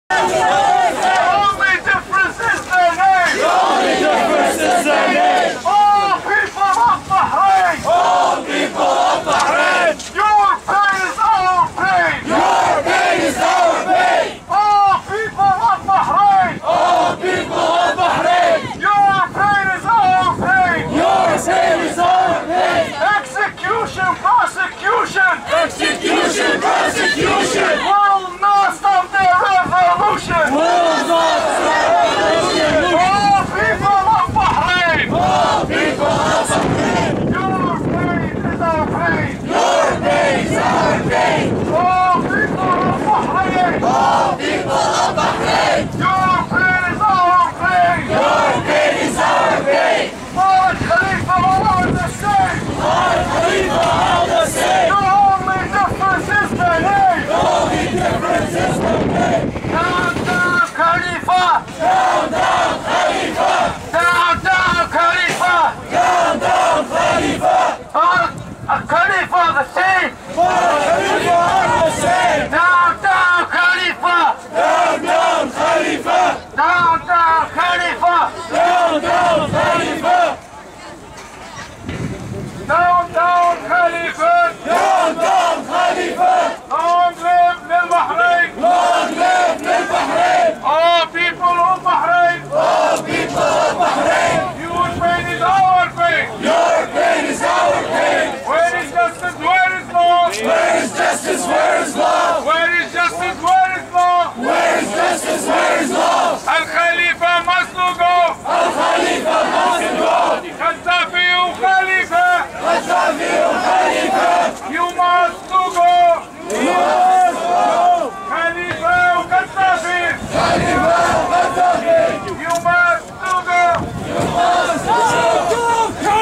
Rally in Ottawa to Support Popular Uprising in Bahrain- Part 2
Rally to Support Popular Uprising in Bahrain held in front of Parliament Hill in Ottawa on Saturday 19, 2011